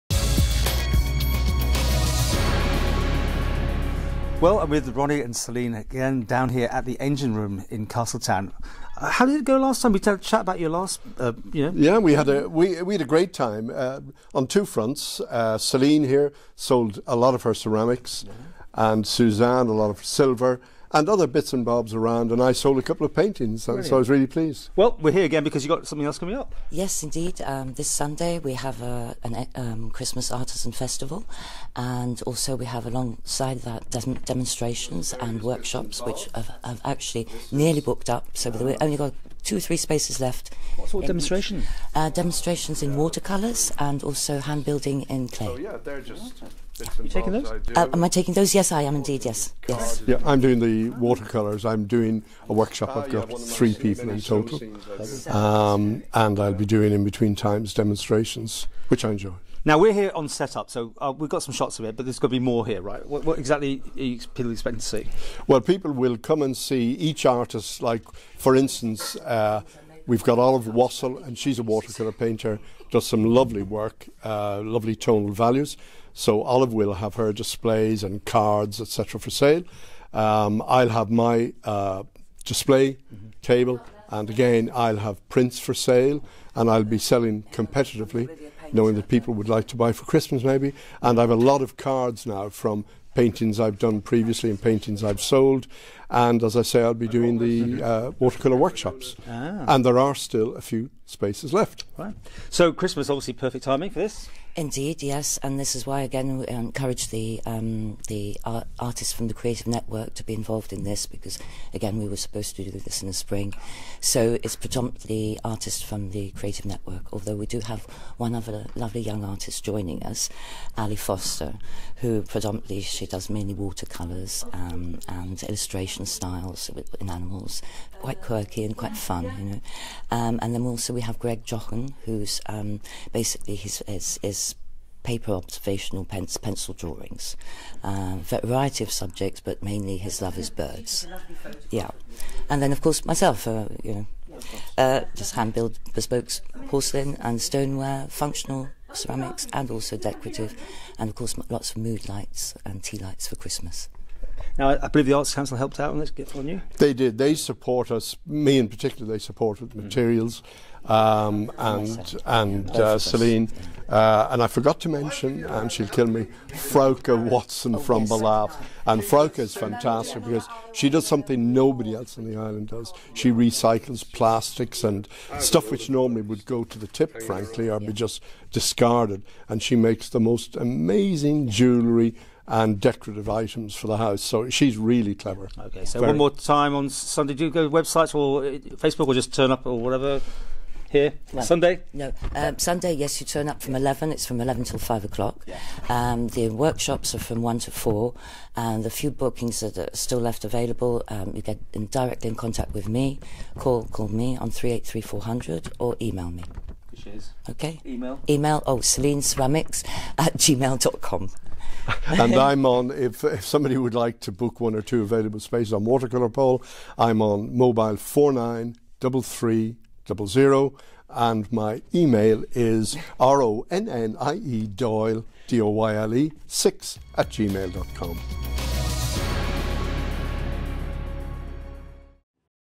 Art exhibition and workshops
Watercolour artist Ronnie Doyle and Ceramic designer Celine Appleyard talk to Paul Moulton about their latest art and workshop event at the Engine House in Castletown.